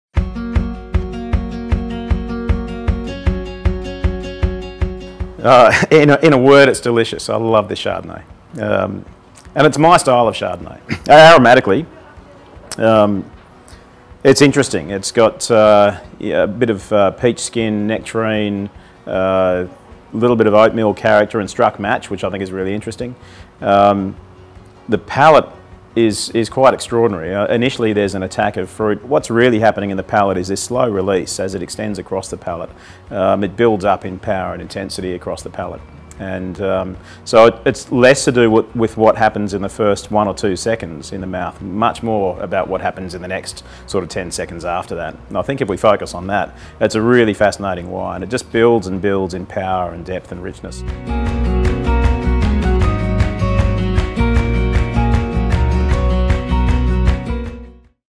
How would you describe this Chardonnay? | Jason Brown
The Wine Society's Best in Society Summer White 2017 is the Moppity Vineyards Barrel Selection Chardonnay 2017.

We had the pleasure of chatting to Moppity Vineyards winemaker and owner Jason Brown about this stunning wine.